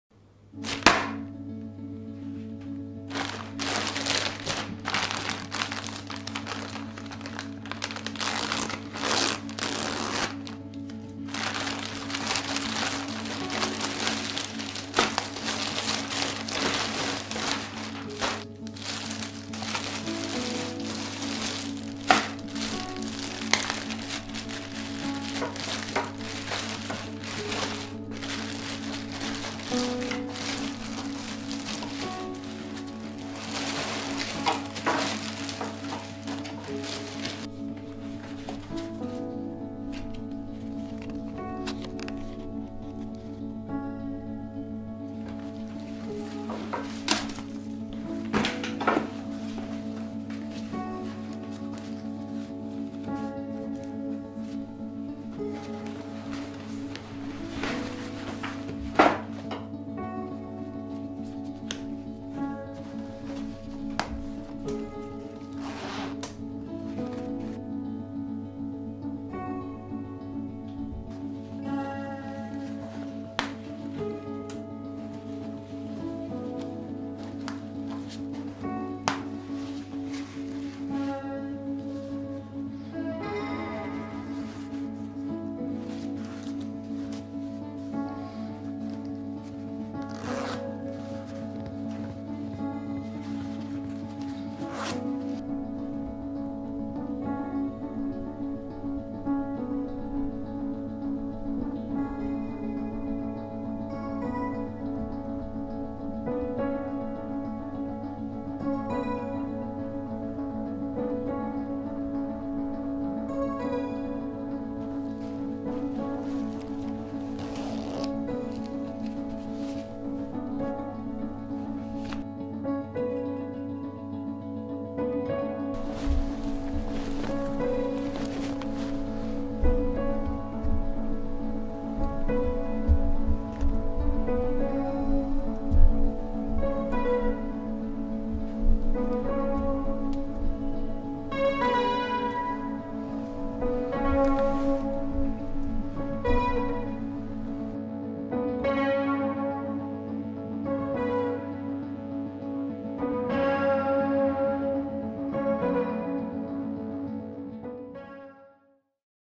Men's Uncharted - Utopia Jacket (unboxing + fit)
Here it is fans, the official Naughty Dog apparel...the Uncharted Jacket from Musterbrand. The quality seems pretty good for a light apparel jacket. The material almost feels like it is waterproof from the texture and smoothness...but I don't think it is from the official description. The jacket I ordered was size: Large